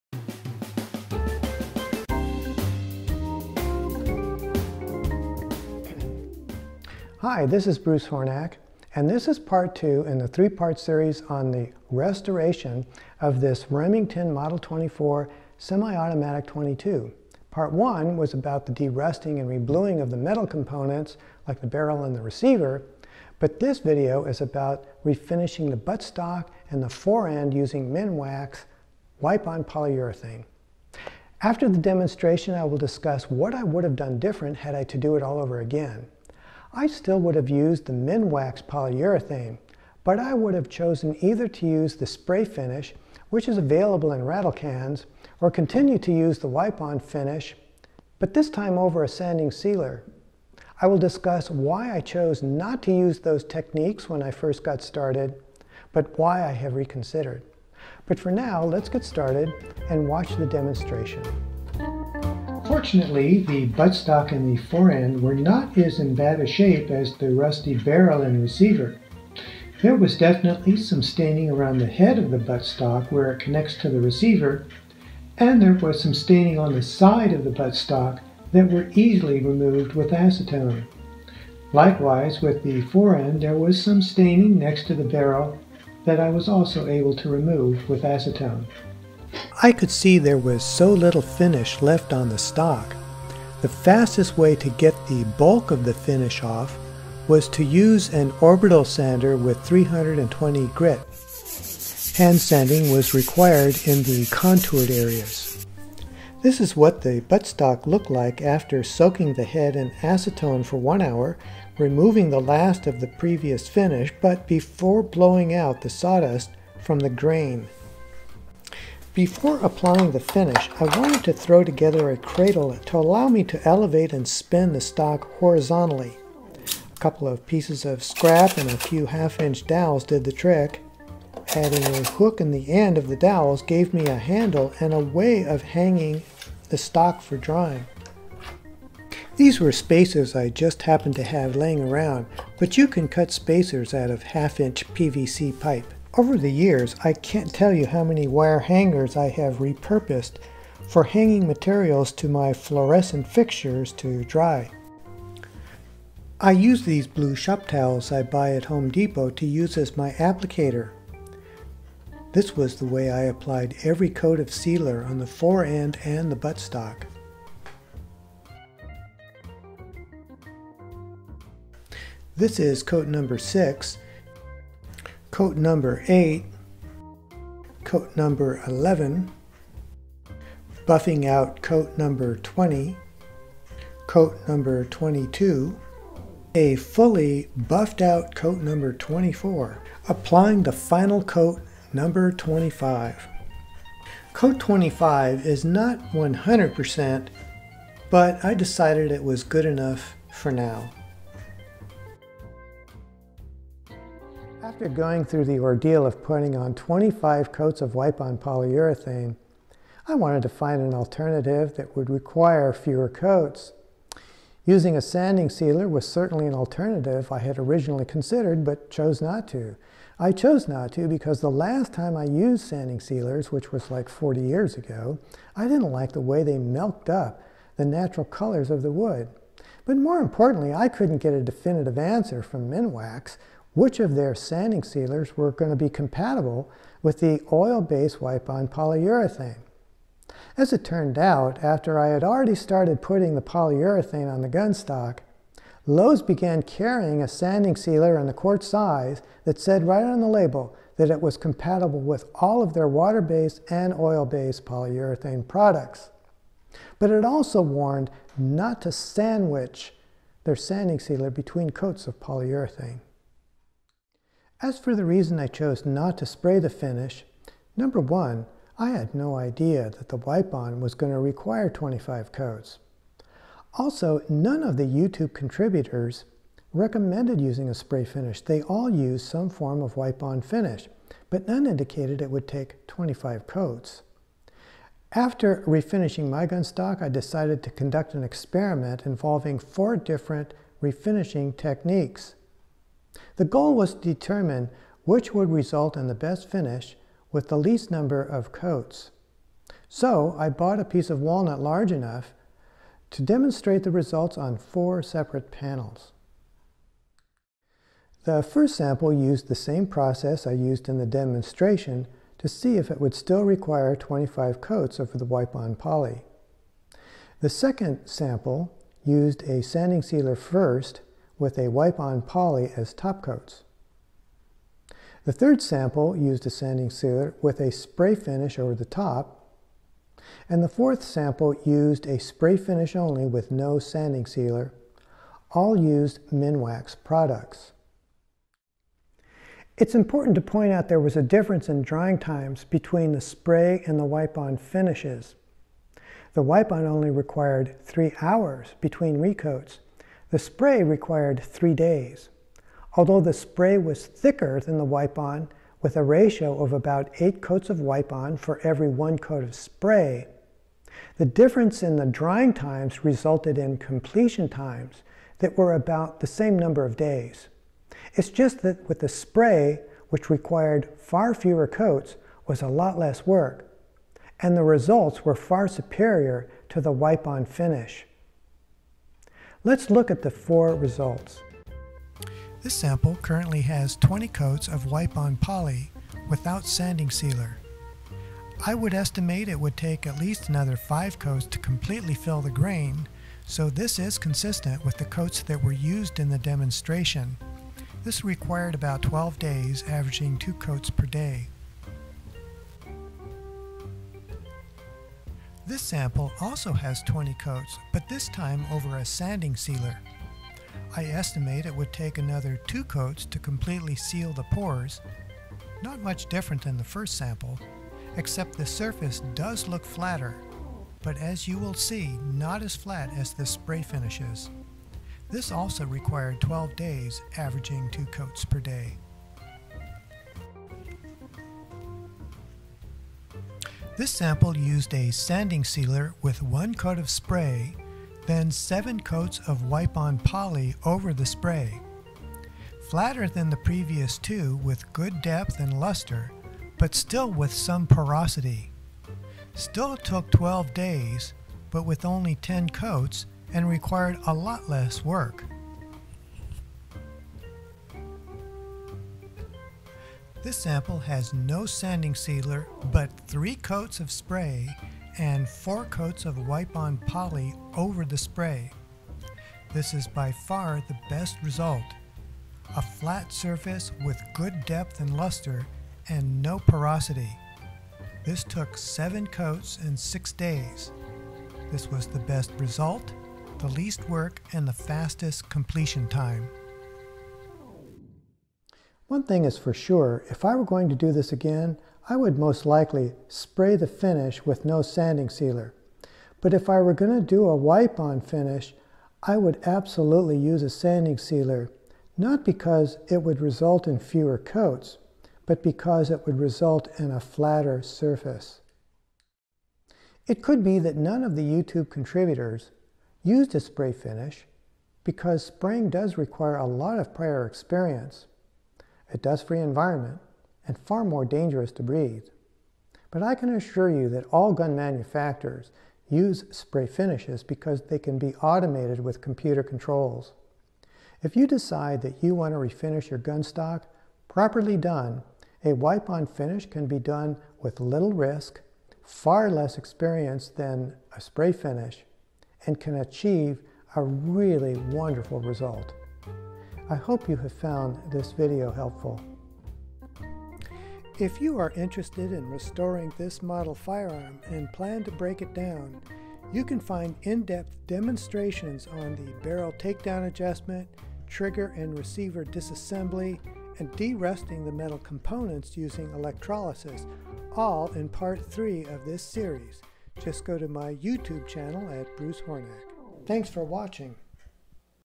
Remington Model 24 Restoration Part 2
Refinish the butt stock and forend using Minwax Wipe-on Polyurethane. Also, a comparison for four different refinishing techniques to determine quality and number of coats required to get a perfect finish.
1.  Spray polyurethane without sanding sealer
2.  Spray polyurethane with sanding sealer.
3.  Wipe-on polyurethane with sanding sealer.
4.  Wipe-on polyurethane without sanding sealer.